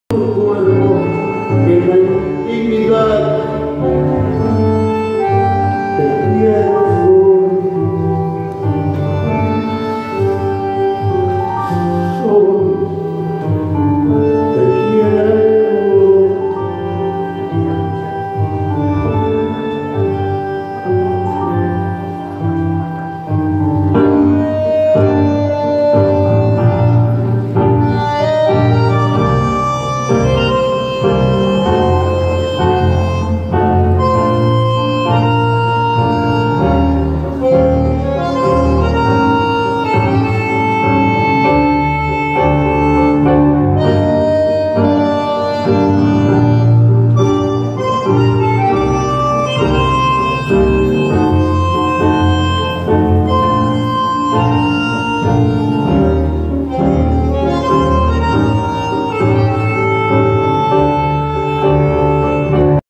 Milonga Misteria, AlterTango Desigual
AlterTango Desigual la prima video milonga in Italia con eventi a tema video proiezioni immersive.

Milonga Misteria, cavalieri mascherati